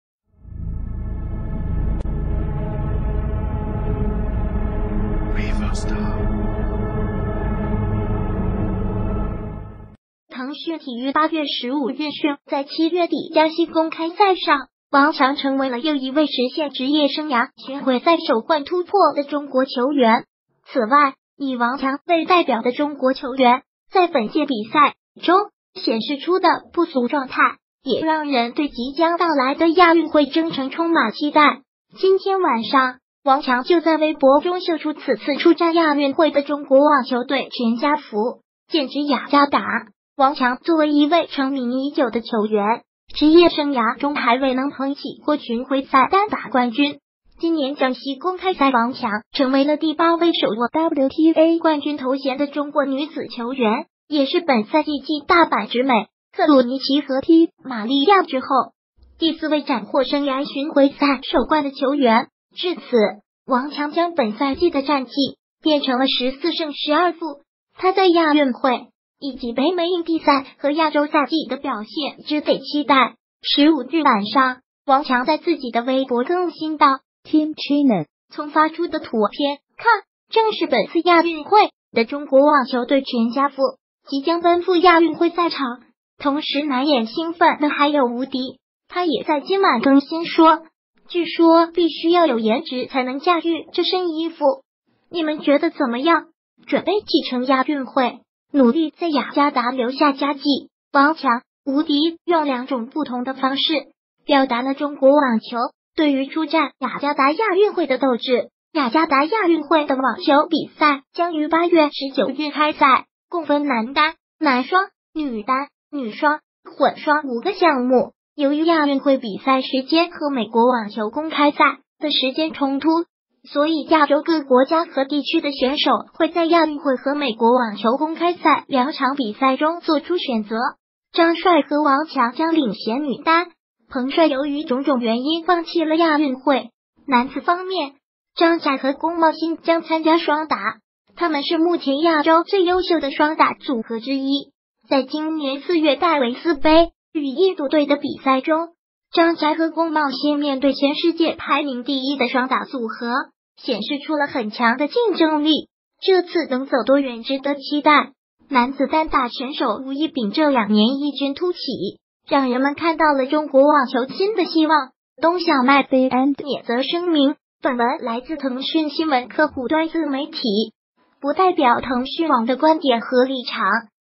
剑指亚运金牌的又一支王者之师 王蔷吴迪发文表决心
)腾讯体育8月15日讯在7月底江西公开赛上，王蔷成为了又一位实现职业生涯巡回赛首冠突破的中国球员。此外，以王蔷为代表的中国球员在本届比赛中显示出的不俗状态，也让人对即将到来的亚运会征程充满期待。今天晚上，王蔷就在微博中秀出此次出战亚运会的中国网球队全家福，剑指雅加达。王蔷作为一位成名已久的球员，职业生涯中还未能捧起过巡回赛单打冠军。今年江西公开赛王蔷成为了第八位手握WTA冠军头衔的中国女子球员，也是本赛季继大坂直美、克鲁尼奇和T-玛利亚之后，第四位斩获生涯巡回赛首冠的球员。至此，王蔷将本赛季的战绩变成了14胜12负。她在亚运会，以及北美硬地赛和亚洲赛季的表现值得期待。15日晚上，王蔷在自己的微博更新道：“TeamCHINA”，从发出的图片看正是本次亚运会的中国网球队全家福。即将奔赴亚运会赛场，同时难掩兴奋的还有吴迪，他也在今晚更新说：“据说必须要有颜值才能驾驭这身衣服，你们觉得怎么样？准备启程亚运会，努力在雅加达留下佳绩！”王蔷、吴迪用两种不同的方式，表达了中国网球对于出战雅加达亚运会的斗志。雅加达亚运会的网球比赛将于8月19日开赛，共分男单、男双、女单、女双、混双五个项目。由于亚运会